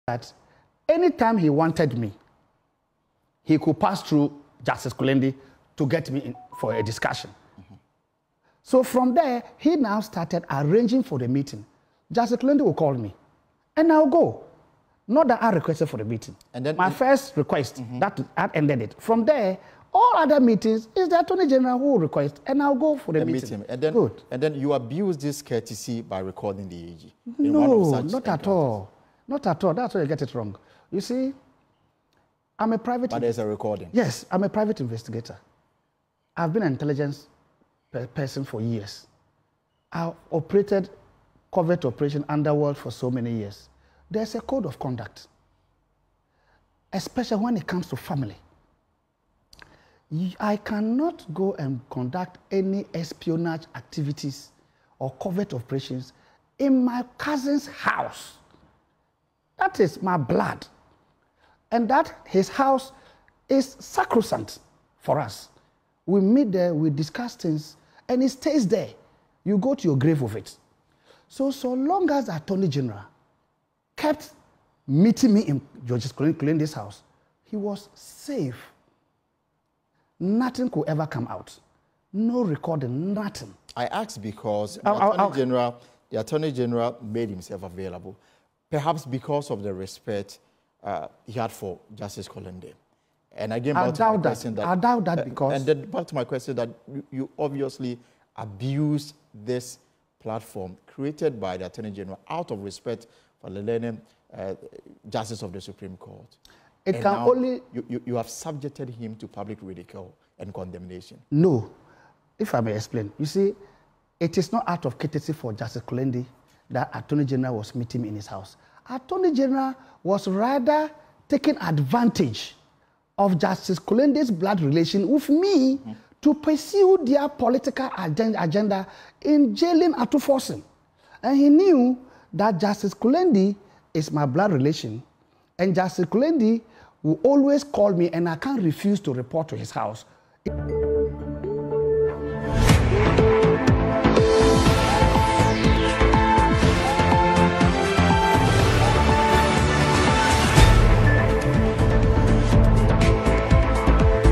Ambulance case: AG exploited Justice Kulendi's relationship with me to jail Dr. Ato Forson - Jakpa
Ambulance case: AG exploited Justice Kulendi's relationship with me to jail Dr. Ato Forson. - Richard Jakpa